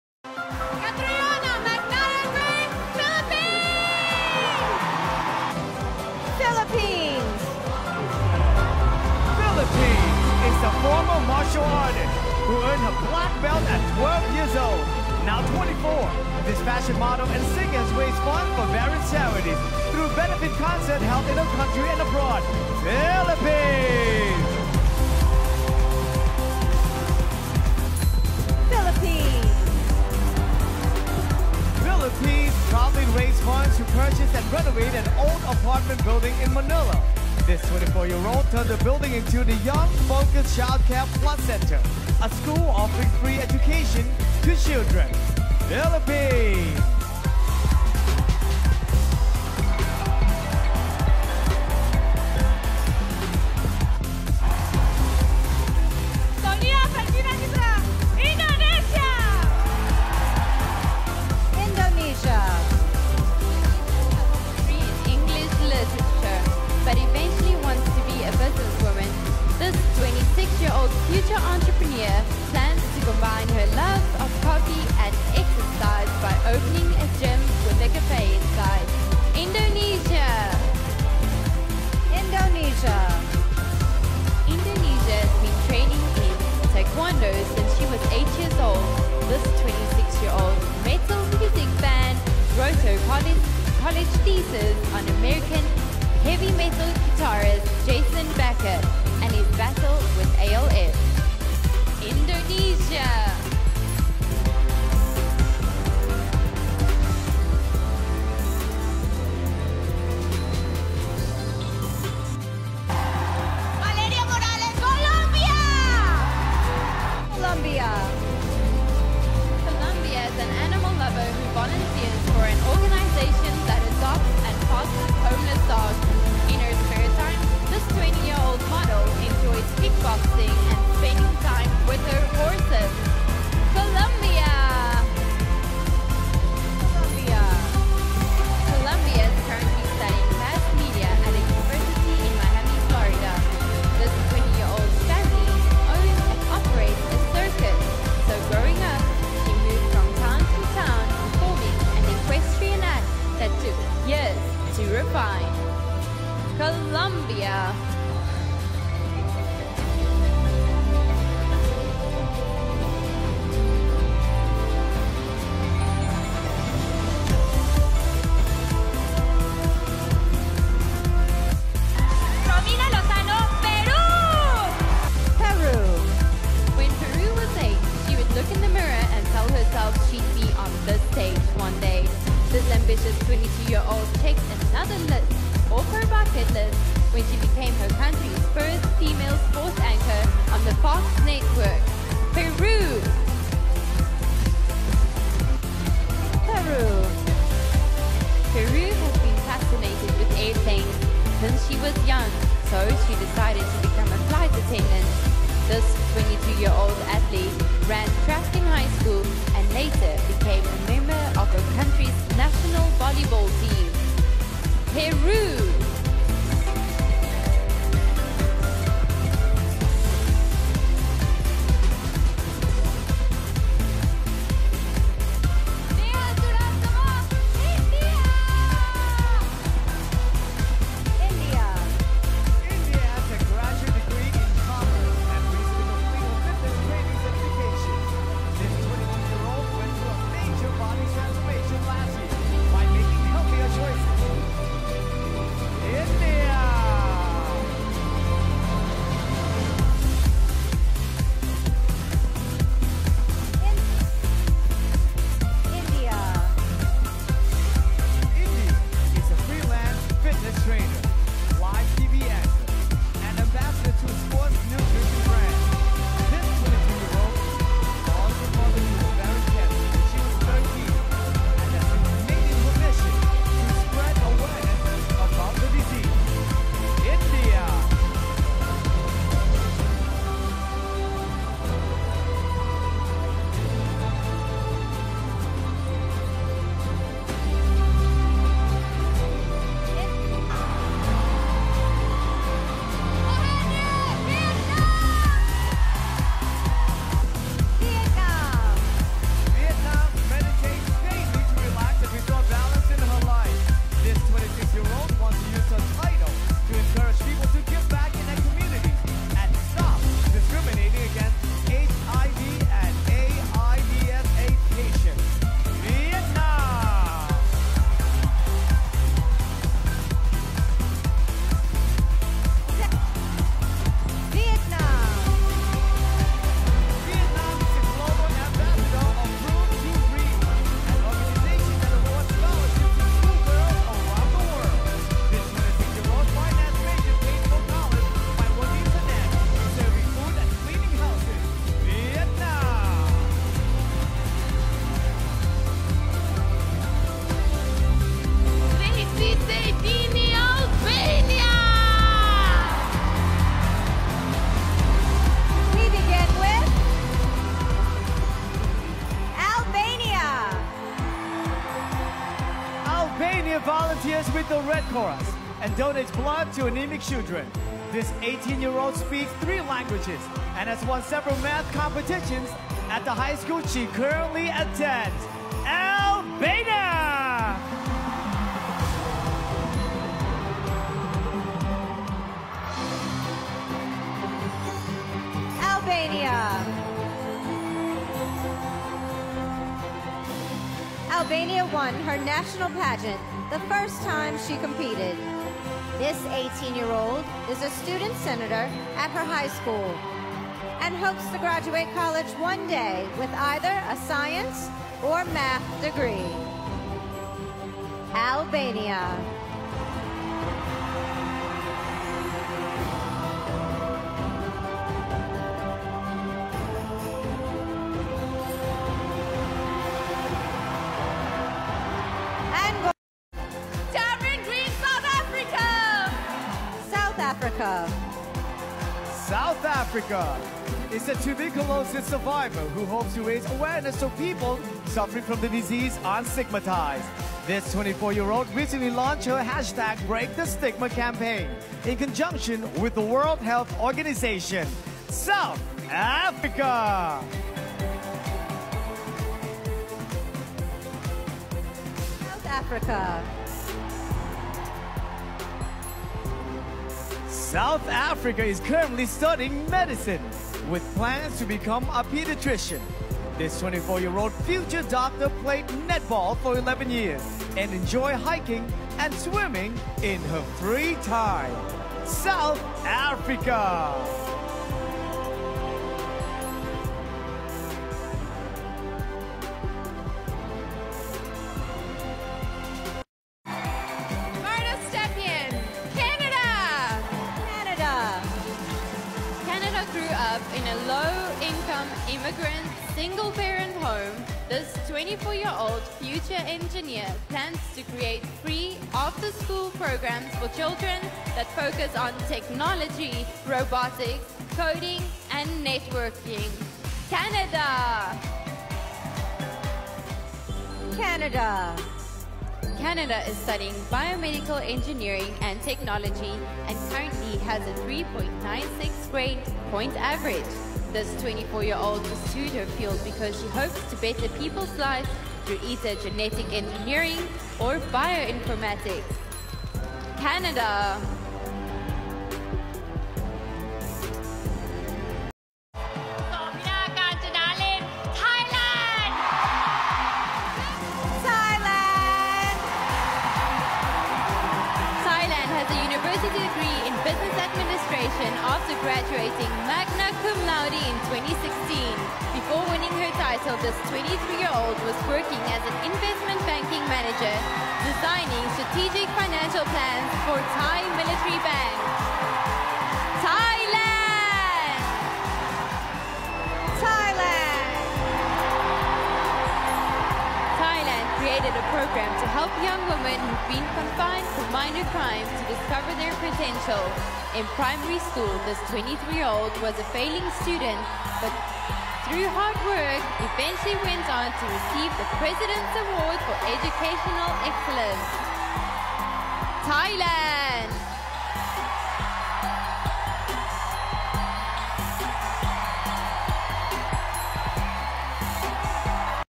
MissUniverse2018 Top 10 Best Performance/Best Catwalk/Best in Swimsuit/ Bangkok Thailand
Swimsuit/Evening Gown/top 1 Catriona Gray//Ang slow lava walk ni Catriona centro ng attraction